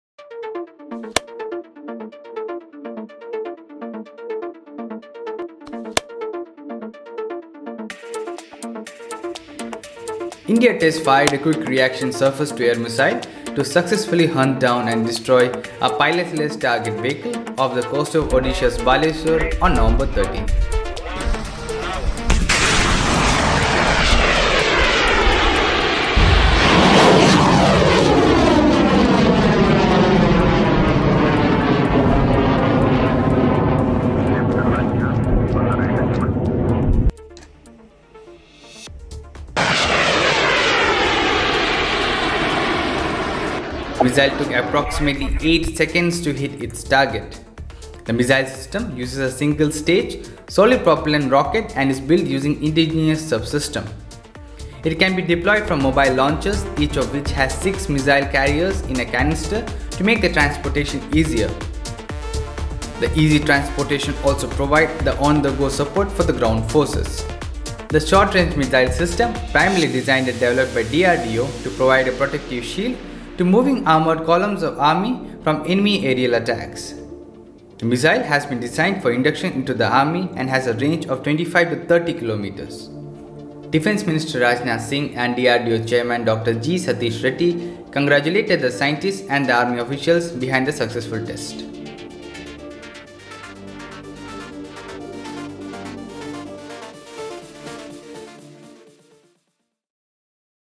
India successfully test-fires Quick Reaction Surface to Air Missile system
The Defence Research and Development Organization (DRDO), carried out a successful test of Quick Reaction Surface to Air Missile system on Friday.